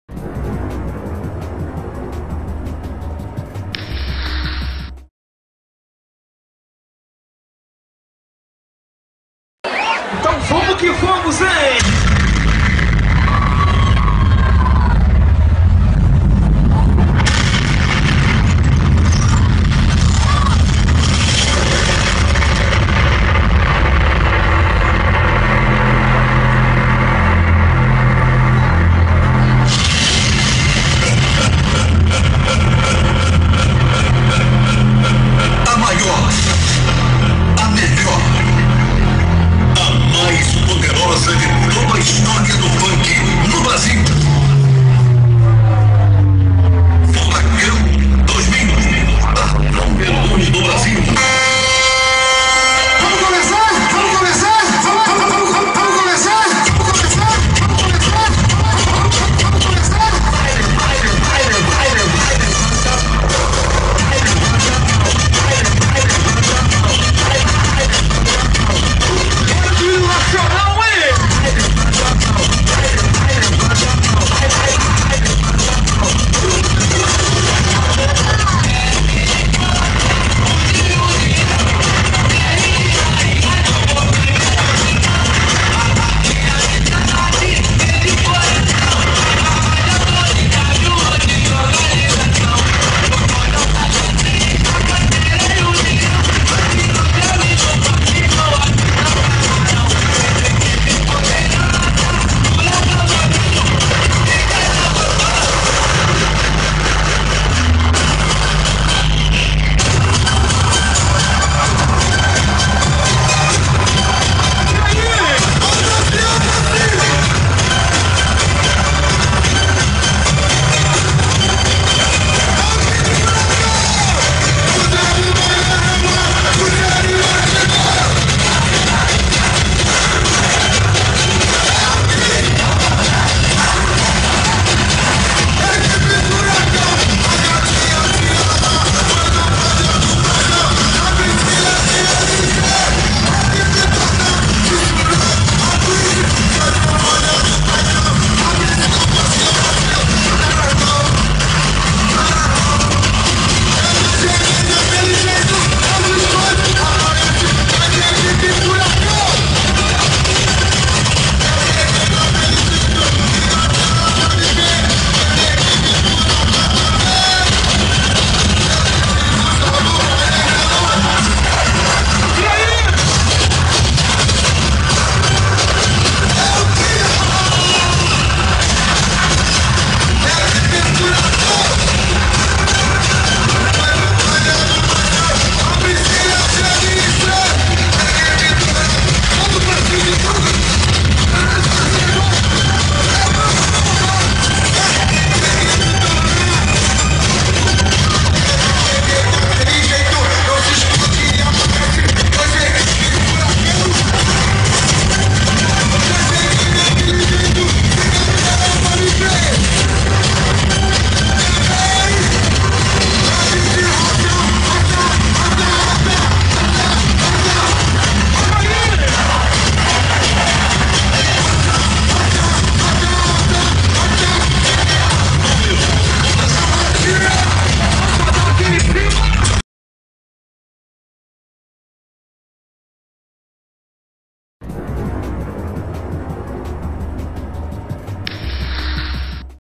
Bandidos fazem baile funk no Morro da Pedreira
Músicas desafiam UPP e lider de facção criminosa
Crédito: Divulgação